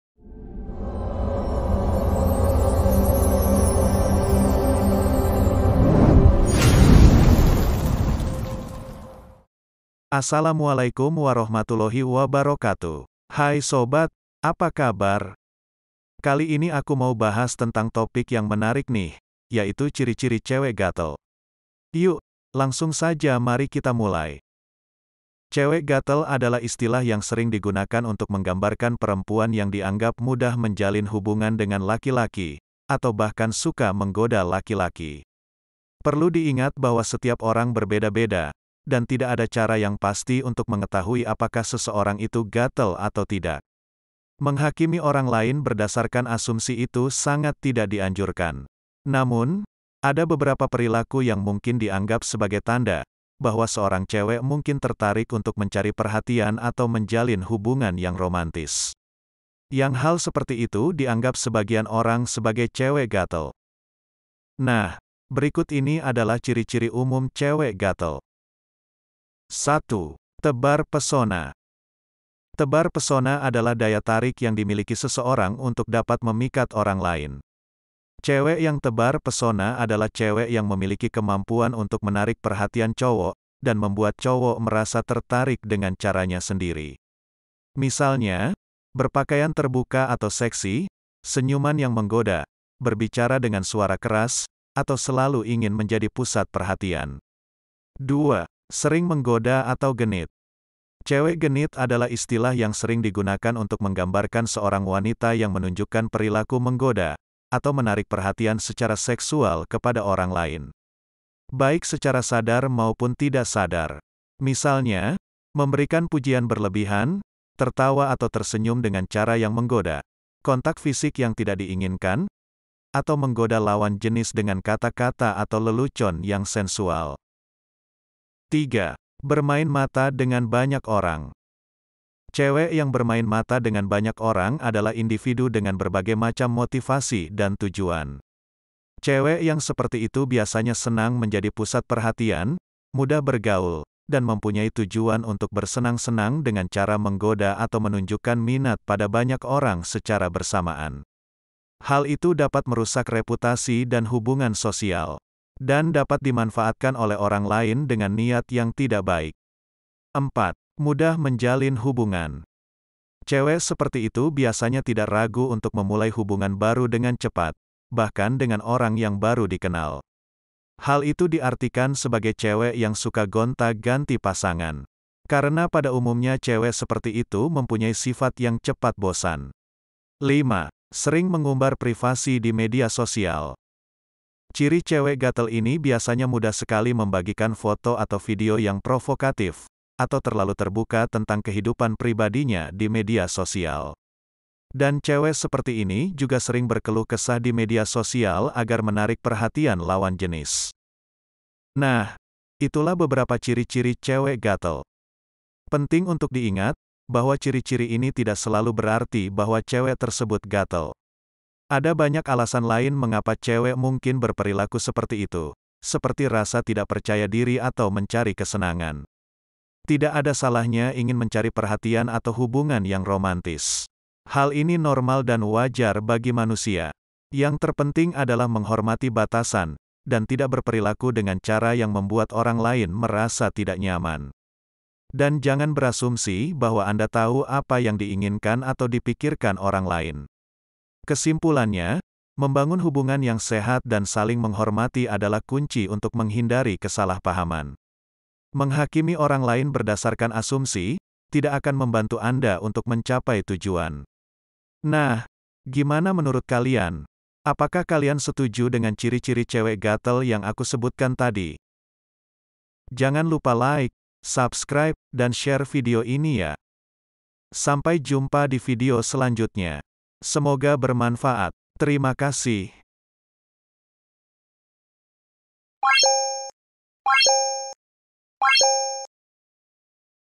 Ciri Ciri Cewek Gatel Dan Genit
Dalam video ini, kita akan membahas tentang ciri ciri cewek gatel dan genit.

Pernahkah kamu mendengar istilah "cewek gatel"? Istilah ini sering digunakan untuk menggambarkan perempuan yang dianggap mudah menjalin hubungan dengan laki-laki, atau bahkan suka menggoda laki-laki. Tapi, apakah istilah ini benar-benar ada? Atau hanya sebuah stereotip yang dilekatkan pada perempuan?

Video ini akan membantu kamu untuk memahami lebih dalam tentang cewek gatel dan bagaimana stereotip ini dapat berdampak negatif pada perempuan.

Video ini hanya bertujuan sebagai informasi  umum dan tidak dimaksudkan untuk menyinggung, menyudutkan atau menyalahkan individu.